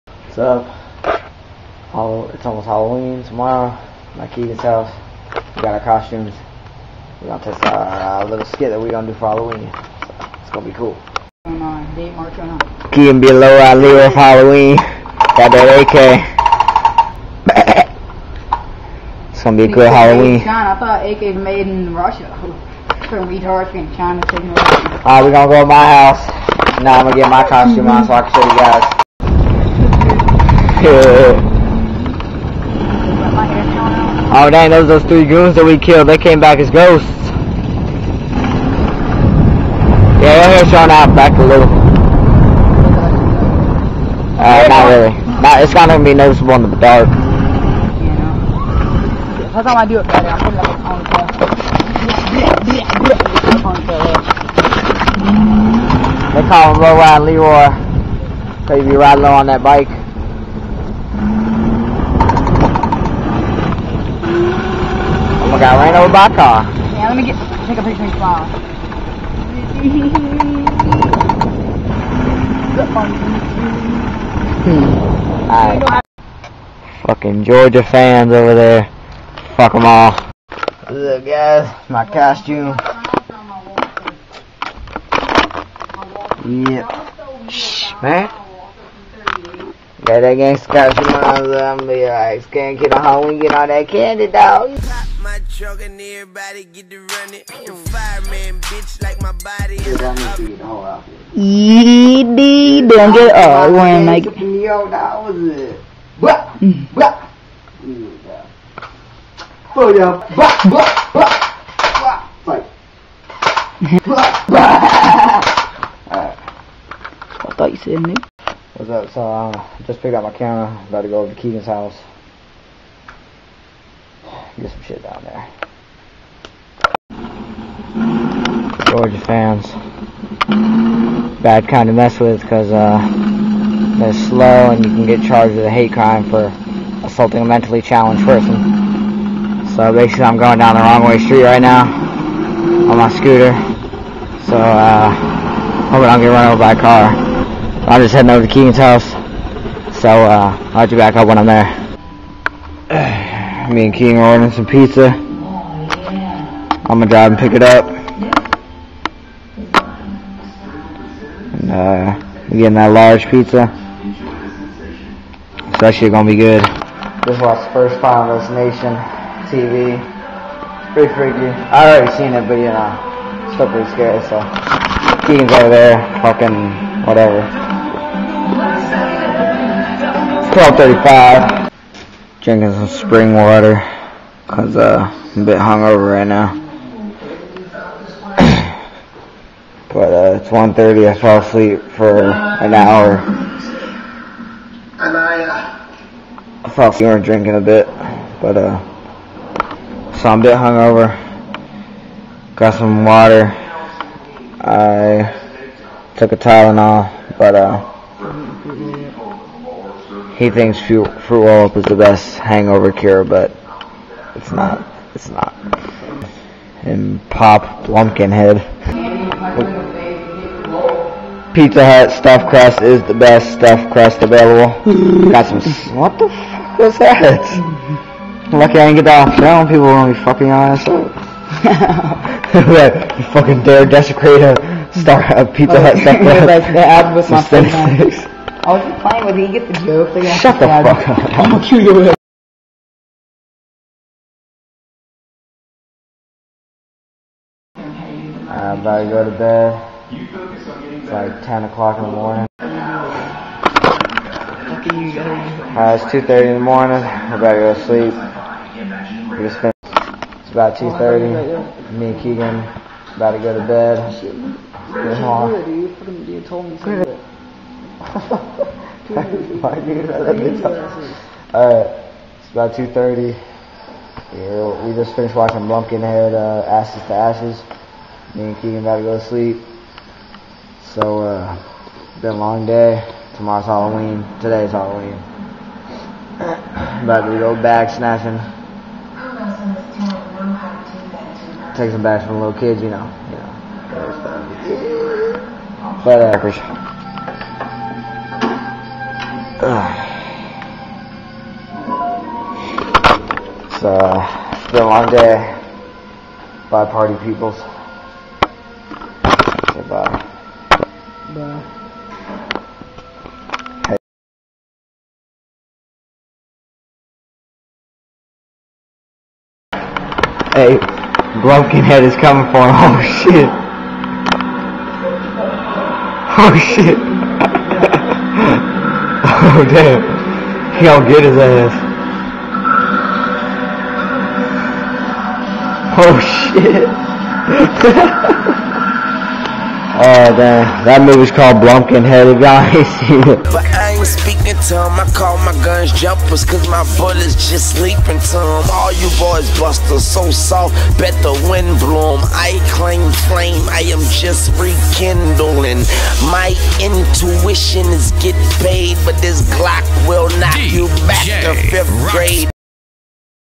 Vlog #2 - Almost Halloween! - (10-30-09)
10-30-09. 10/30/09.

Me and keegan gettin our costumes ready and practicing for the skit were gonna do at the halloween party down the street. We're getting $300 each to perform at a kids party next week! We are getting $50 for this halloween party 10-31-09.
Keegan is going as low ridin leroy and im going as a cubano gangster.